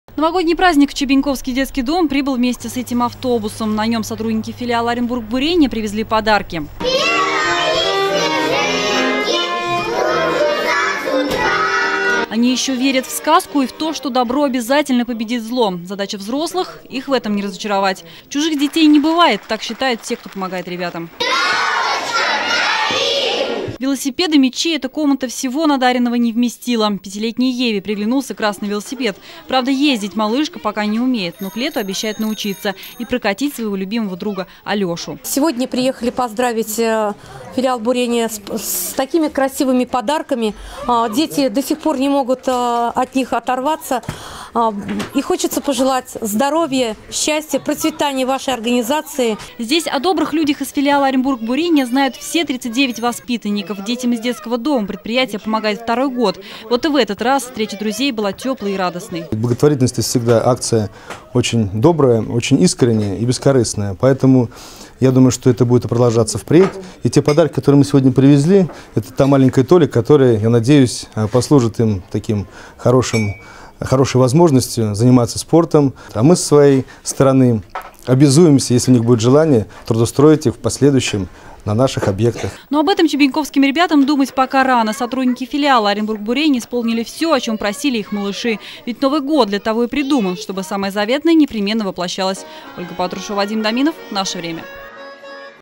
27-12-12 От души
Новый год уже пришел и в Чебеньковский детский дом. Море тепла и гору подарков ребятам сегодня привезли сотрудники оренбурского филиала компании «Газпром бурение». Мячи, велосипеды, тренажеры — надаренное пришлось доставлять на большом автобусе. Удивить ребят старались все работники предприятия. А вот получилось ли, знает Ольга Патрушева.